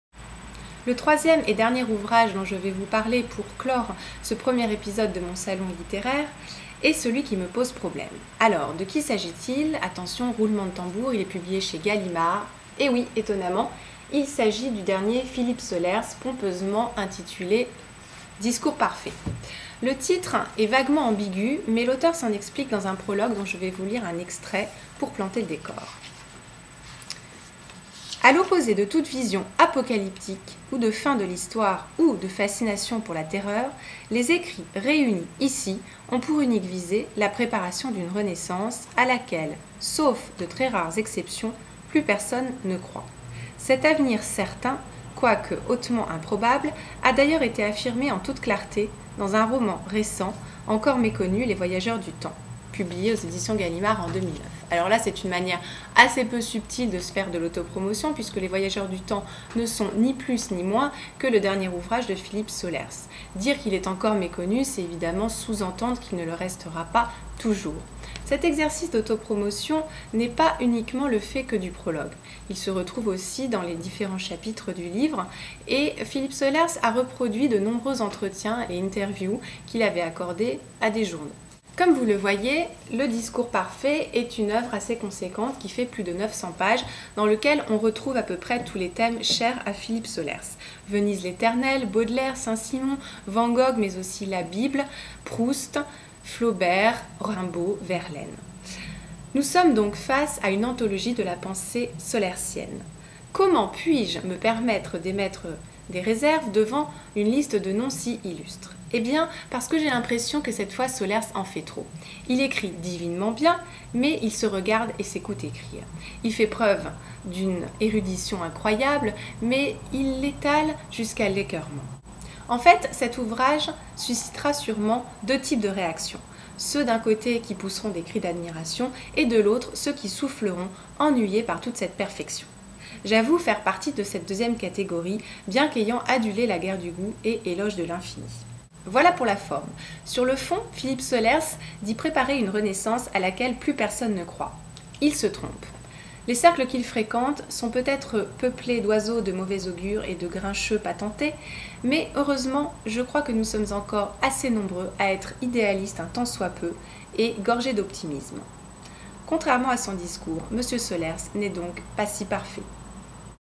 "Discours parfait" de Philippe Sollers
Le Journal de Nathalie Six - Mon salon littéraire - Episode 1 (janvier 2010), "Discours parfait" de Philippe Sollers aux éditions Gallimard